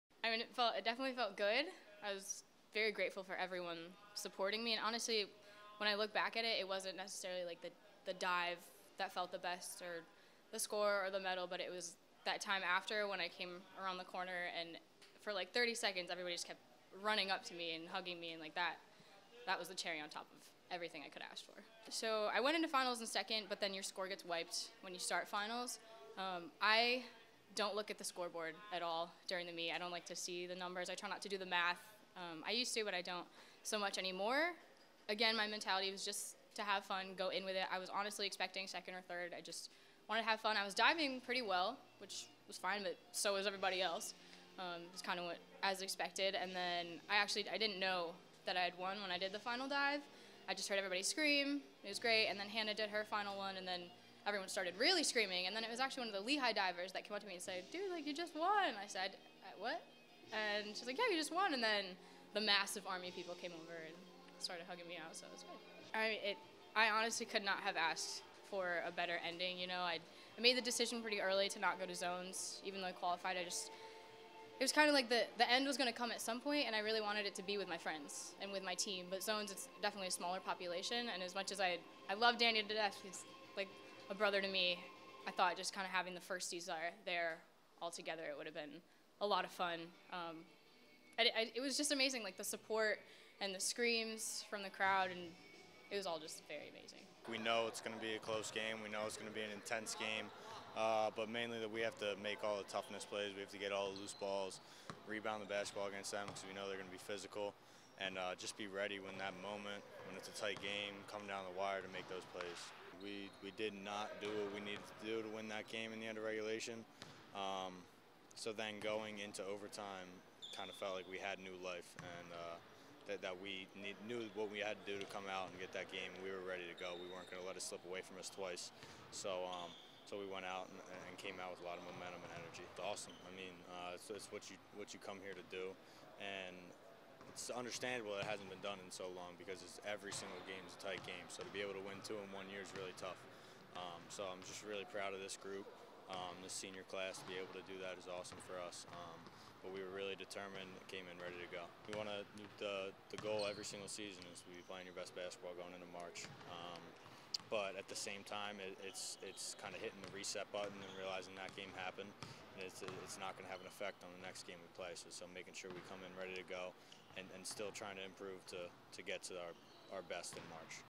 Co-Athletes of the Week: Serica Hallstead and Tommy Funk
Serica Hallstead won the 1-meter diving championship and Tommy Funk led Army to a star victory over Navy.